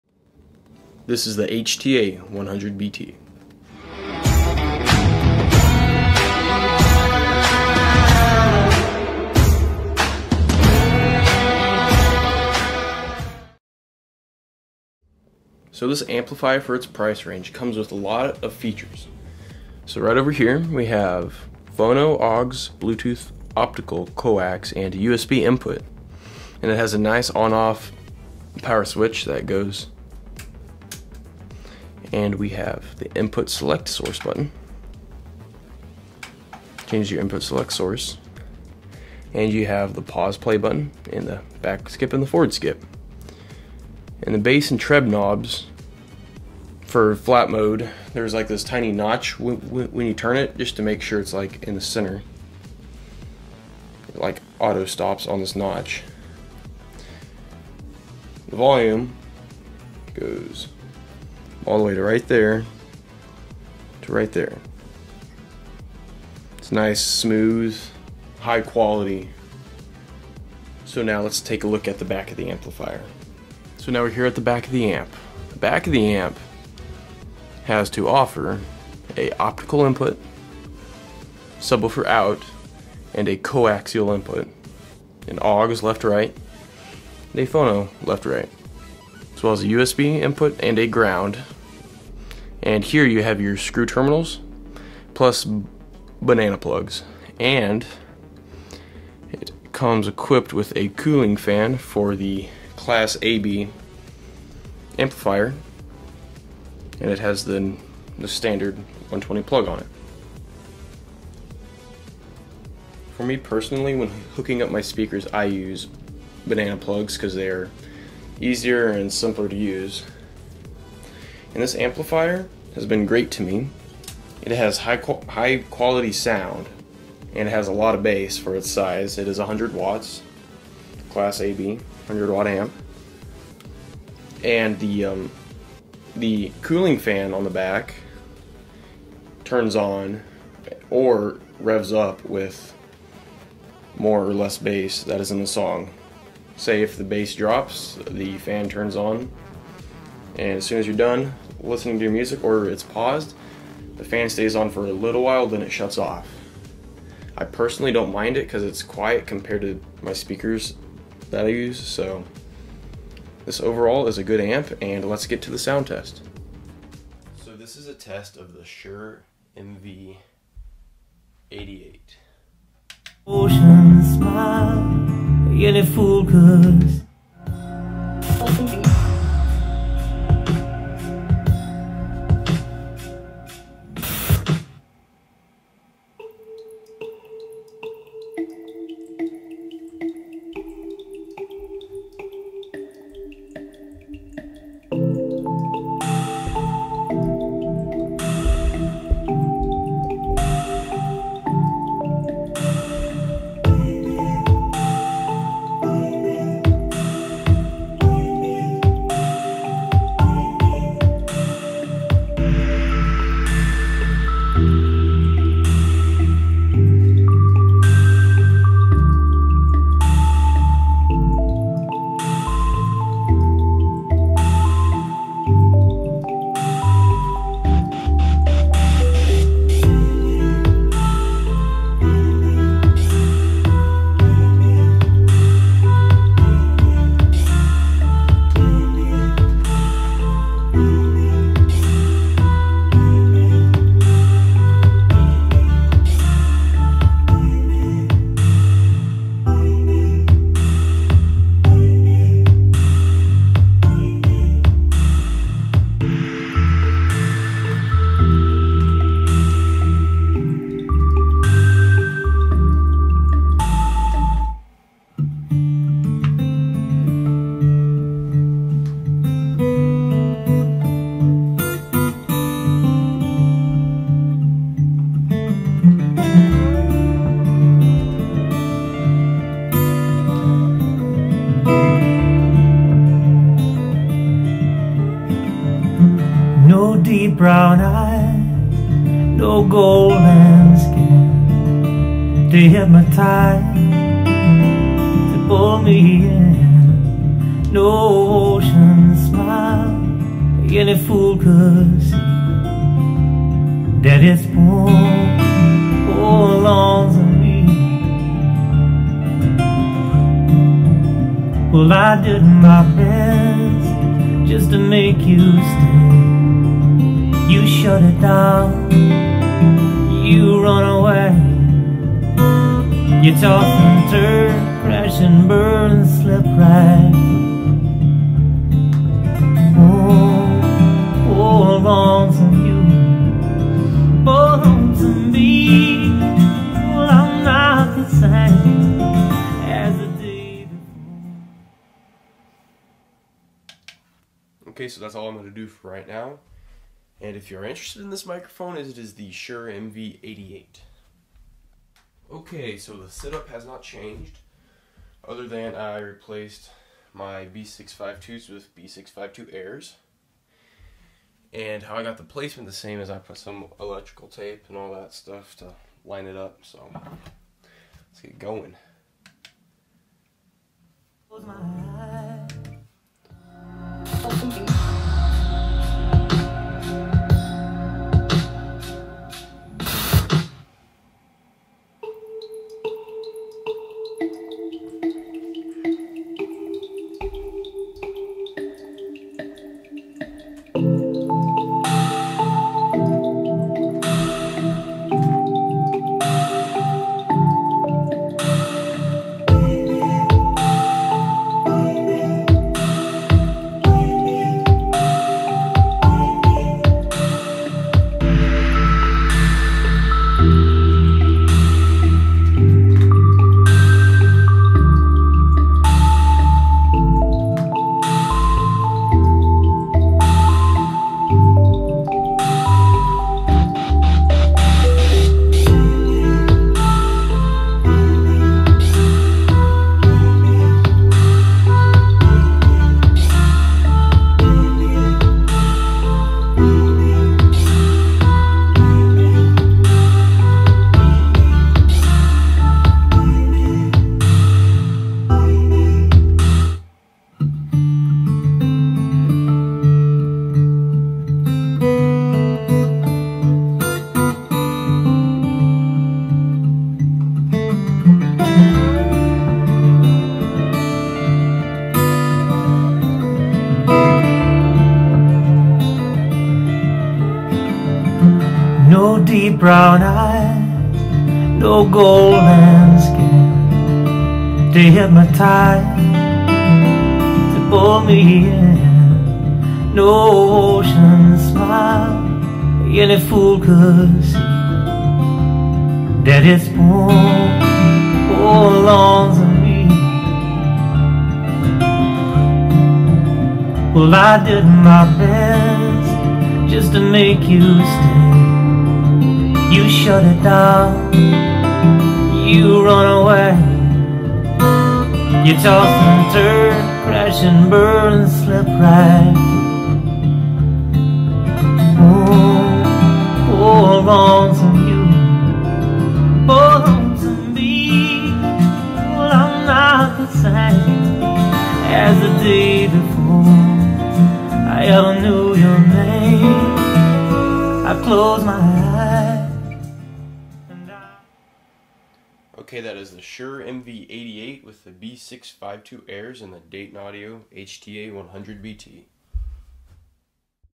Dayton Audio HTA100BT Review + B652/B652-AIR Audio Test
00:00 Intro
00:16 Review
2:46 B652 Sound Test
5:52 B652-AIR Sound Test